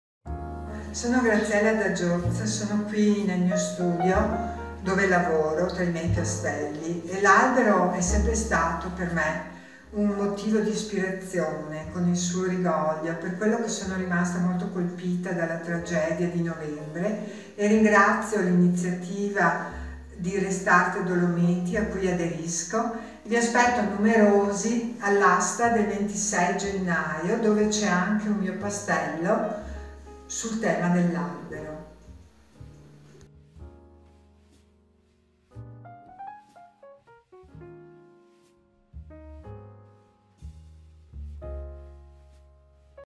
RESTARTDOLOMITI - Graziella Da Gioz
Albero come fonte di ispirazione. Lo è sempre stato nella sua lunga carriera, lo è tanto più oggi, dopo la strage di boschi provocata dall’uragano dello scorso 29 ottobre. È così che la pittrice Graziella Da Gioz, bellunese d’origine e trevigiana di adozione, ha deciso di aderire al progetto RESTARTDOLOMITI con un quadro, realizzato con la tecnica del pastello, che rappresenta proprio un albero. Un’opera decisamente simbolica che vuol essere, secondo gli intendimenti dell’autrice, emozionale. Capace di trasmettere, in qualche modo, il dolore per le migliaia di metri cubi di piante che invece di innalzarsi al cielo ora giacciono a terra.